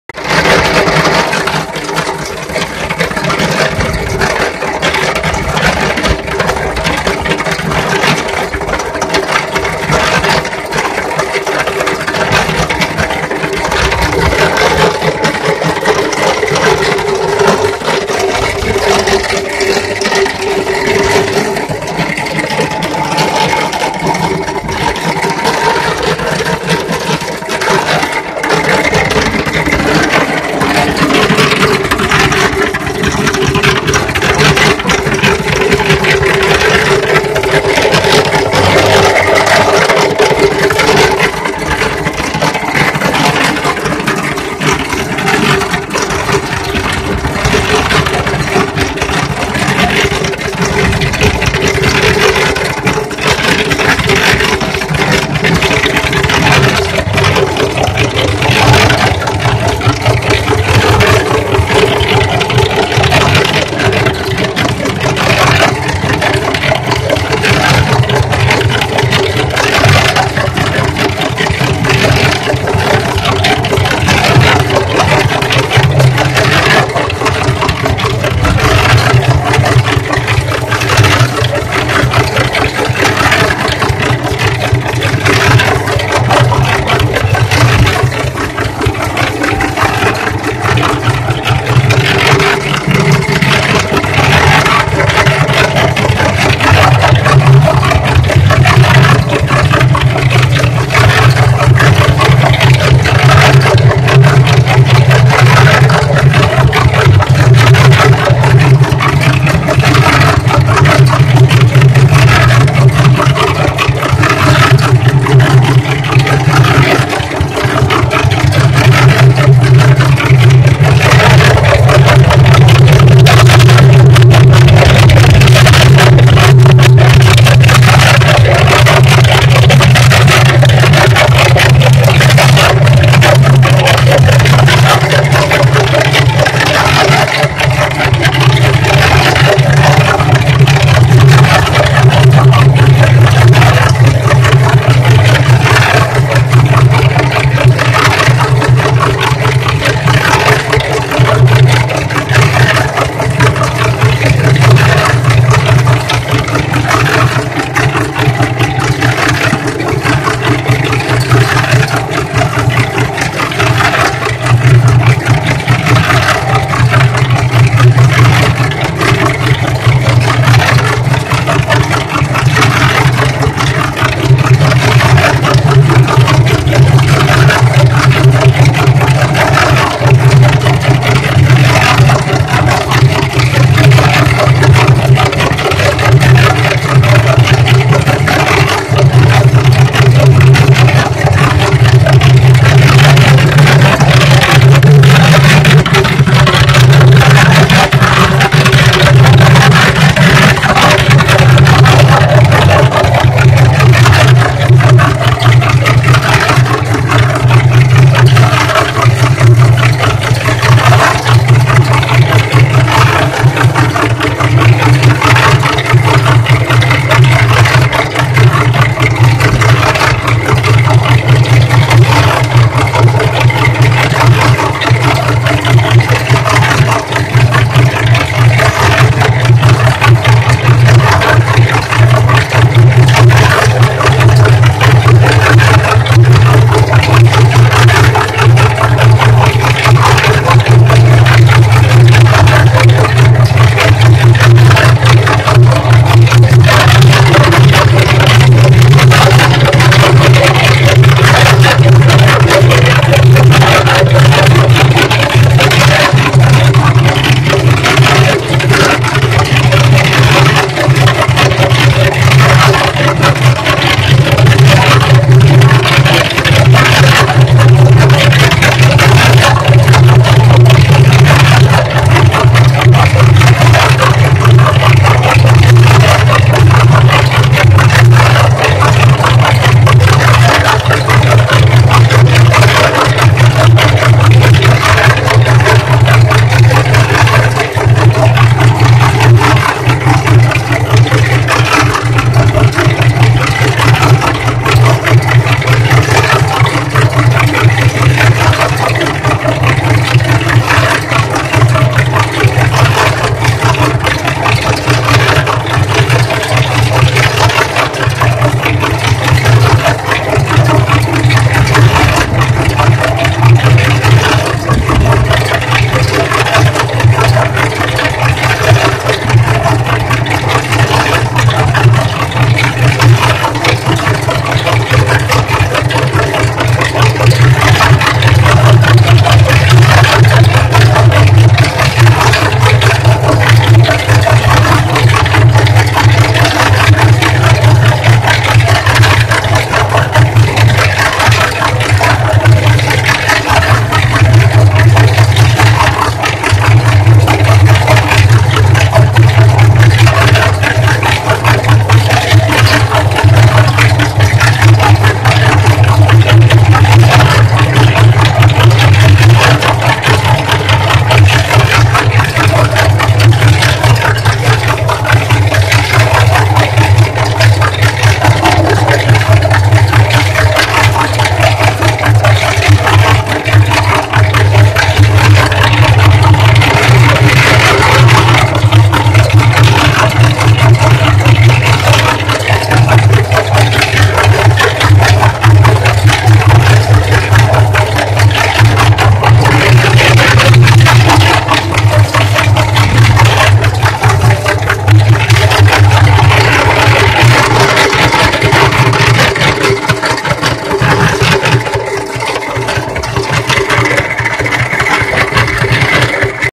Washing Machine From Cement Mixer
Today of all days the Washing Machine Broke Down so I made one from an old Cement Mixer that turned out to work so well that we will be using this instead of buying a new Washing Machine